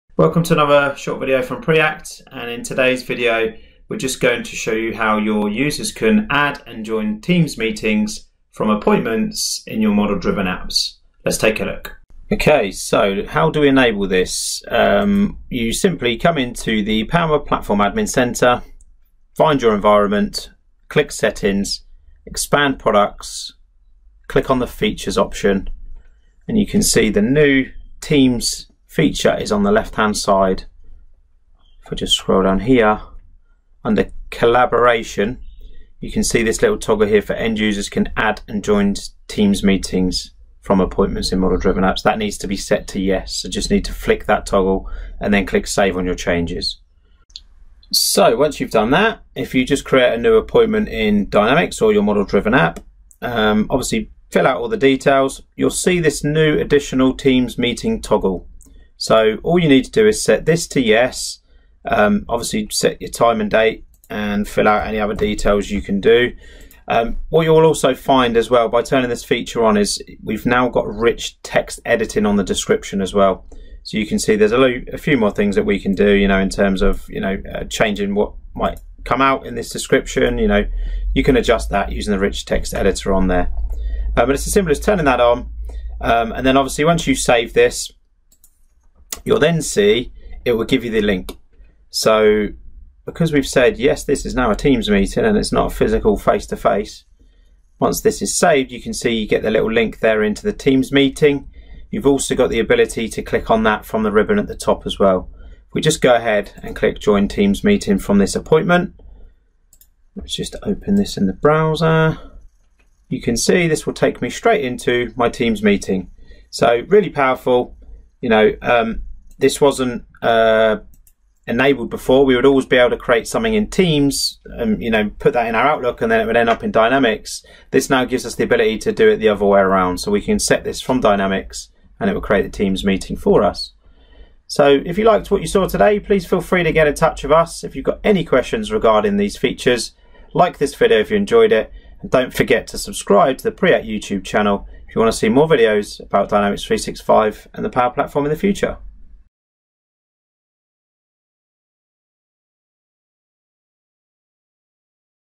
How to Create and Join Teams Meetings from Appointment Records in Dynamics 365
Previously, users would have to create a meeting in Microsoft Teams, add it to Outlook calendars, to then link to appointment records in Dynamics 365.  Now, users can simply use a toggle switch to turn an appointment into a Teams meeting, within Dynamics 365 and other model-driven apps, and join these meetings without having to switch apps.

This is feature is being released alongside a new rich text editor for appointment descriptions, which you can use to link text, create lists and use other rich text elements such as different fonts.